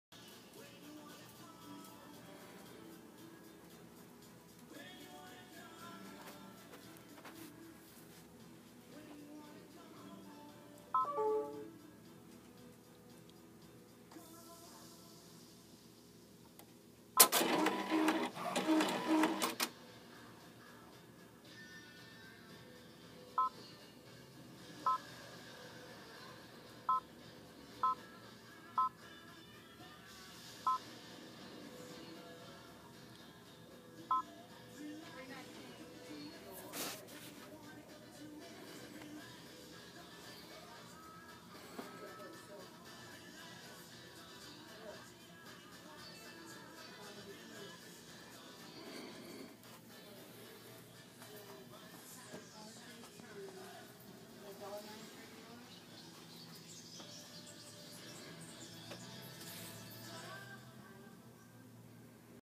Quick look at PR 1000e Entrepreneur Pro 10 needle embroidery machine.
Here is a quick look at the Brother Embroidery Machine PR1000e 10 Needle.